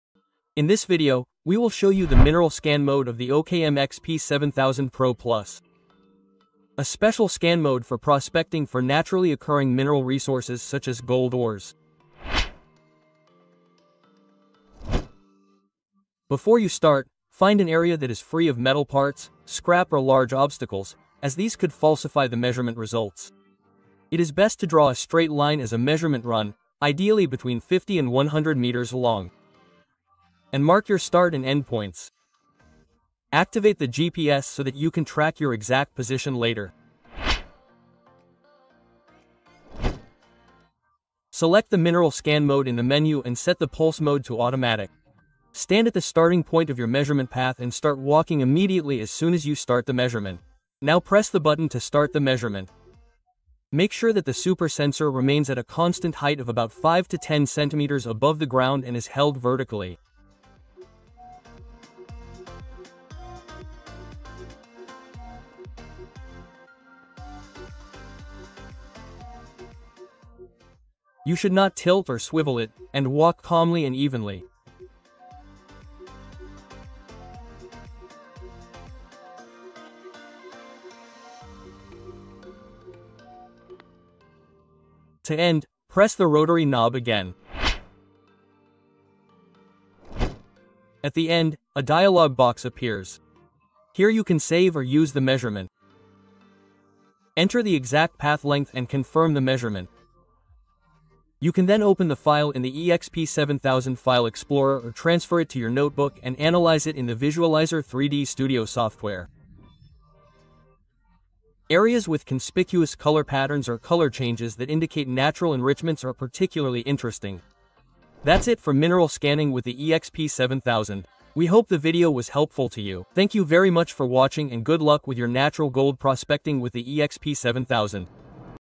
💎 OKM eXp 7000 Professional Plus – Discover Nature’s Hidden Treasures! 🌍🔦
Unveil the secrets beneath your feet with the Mineral Scan feature of the OKM eXp 7000 Professional Plus!
This powerful 3D ground scanner detects natural gold deposits and mineralized zones deep underground — now with a new percentage display showing the potential concentration of minerals in real time.

Perfect for gold prospectors, geologists, and treasure hunters who want precision, depth, and reliability in every scan.

⚙️ Key Highlights:
✅ Detects natural gold and mineralized areas underground
✅ New % display for mineralization intensity
✅ German-engineered technology for unmatched accuracy
✅ Ideal for professional exploration and gold prospecting

🌟 Turn exploration into discovery with OKM’s most advanced ground scanner yet!

📩 Get in Touch for Details!